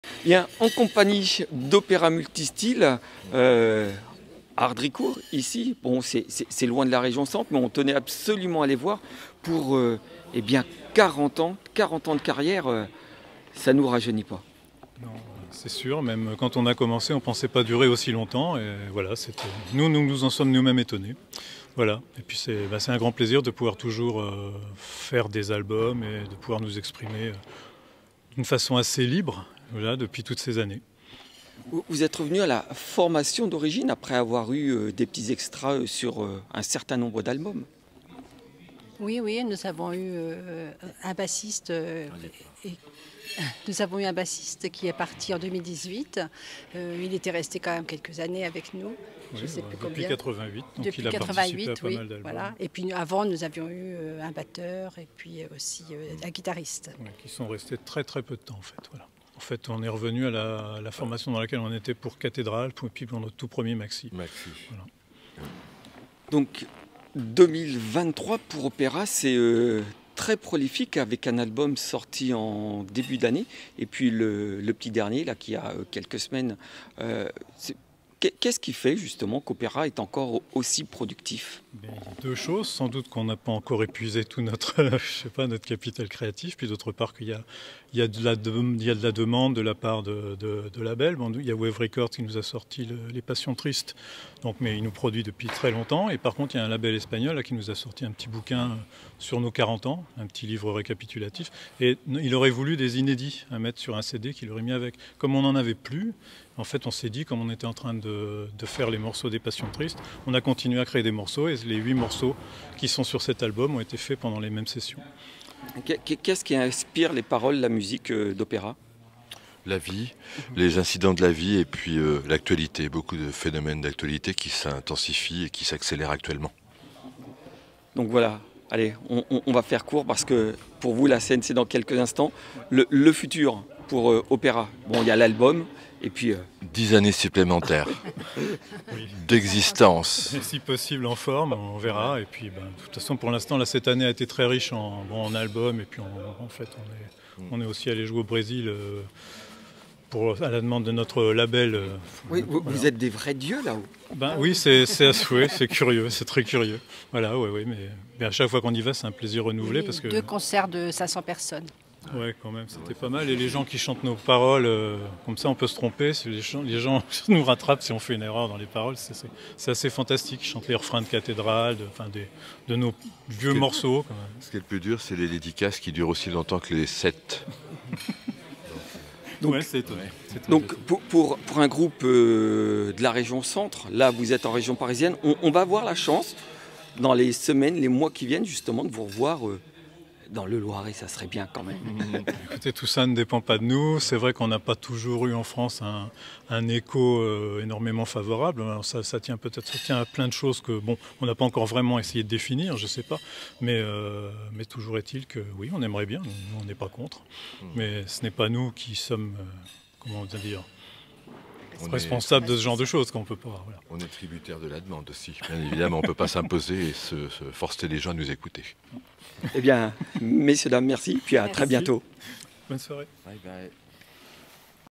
Opéra Multi Steel - Prélude interview - Fêtes Komité 1
Petite interview de « Opéra Multi Steel » réalisée avant son concert à Hardricourt pour « Fêtes Komité 1 » le 25 novembre 2023.